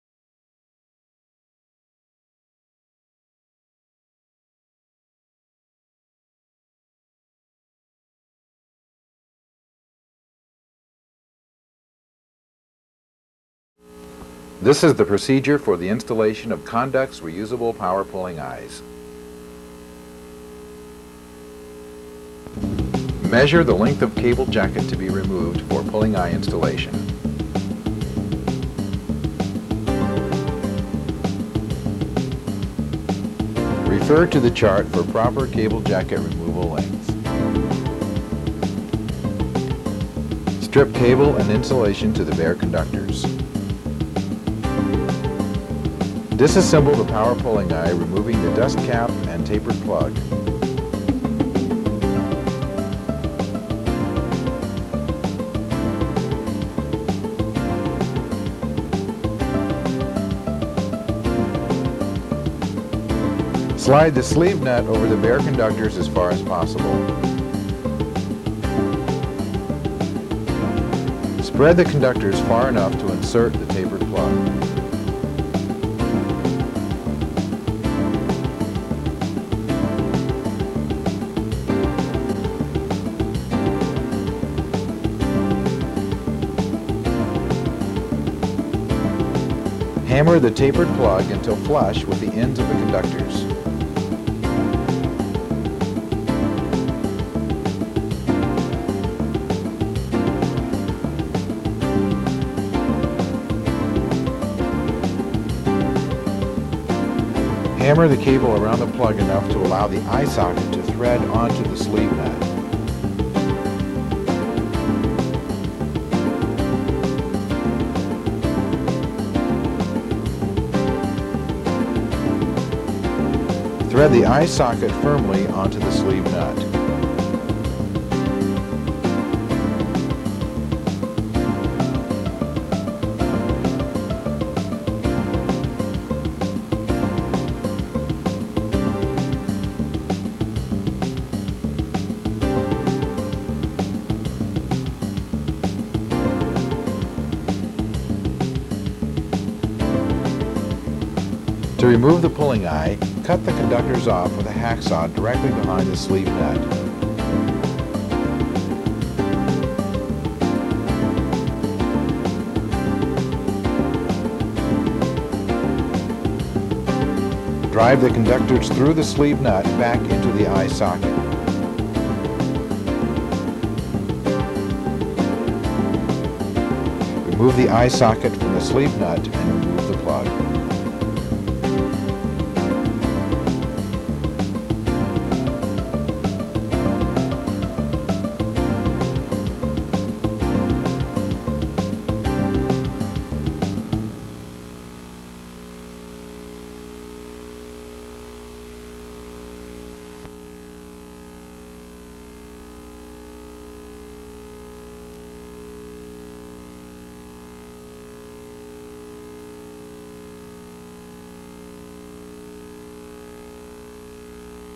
CONDUX - Reusable Power Pulling Eyes Installation Procedure
This video covers both installing and removing Condux reusable power pulling eyes. Watch the step-by-step process of measuring and cutting cable jacket lengths, stripping cable, and disassembling and installing the Condux Reusable Power Pulling Eye, as well as how to remove the Power Pulling Eye when done so it can be reused again for future projects.